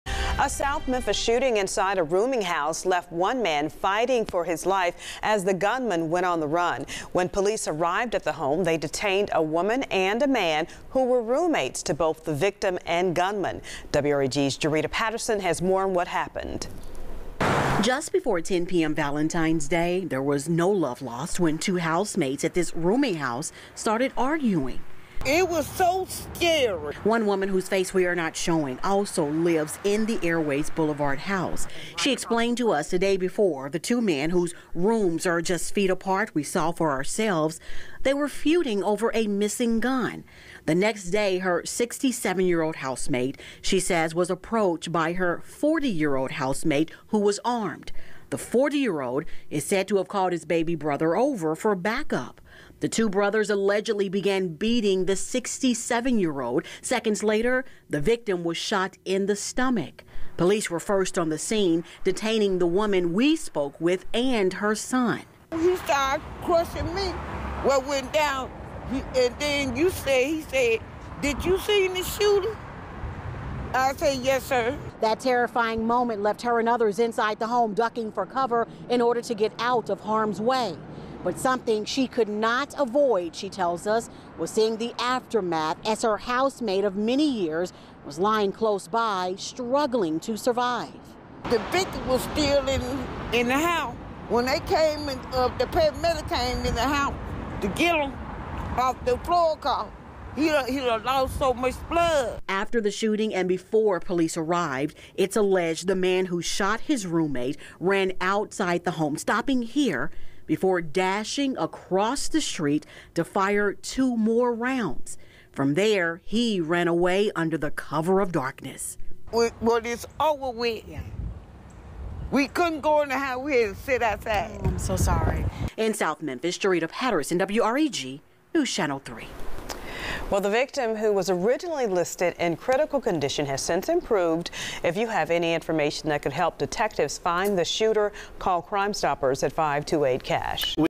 Feud over gun leads to shooting at South Memphis rooming house
A shooting inside a South Memphis rooming house left one man fighting for his life as the suspect went on the run.